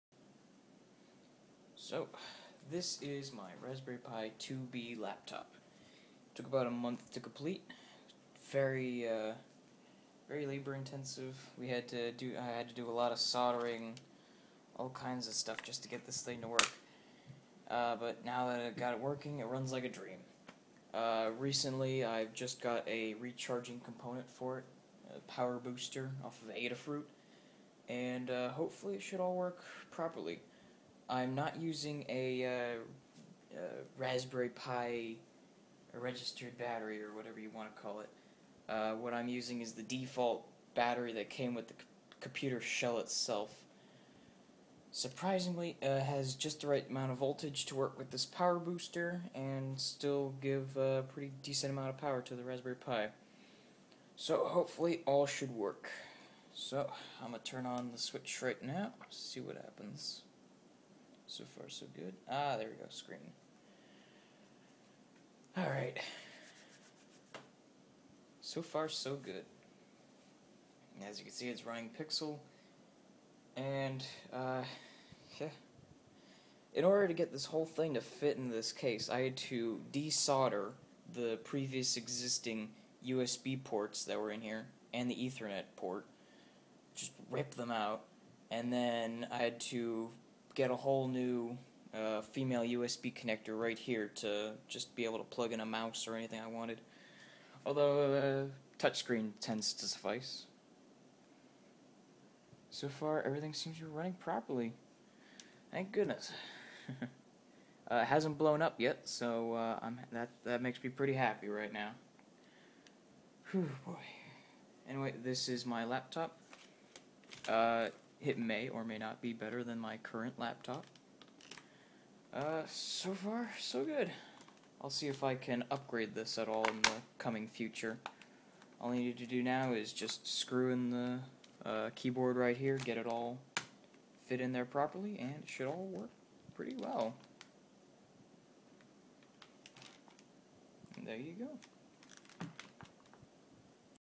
My Homemade Raspberry Pi Laptop
This is my first attempt at creating my own laptop. I think it worked pretty well. There's a few kinks to work out, but all in all I had a great time building it. I know the video quality is pretty bad and you cant see the screen very well, but please bear with it, I'm using an IPhone 3gs to record. I may upload a new video sometime in the future explaining how it works and it's specs, but till then. Hope you enjoy!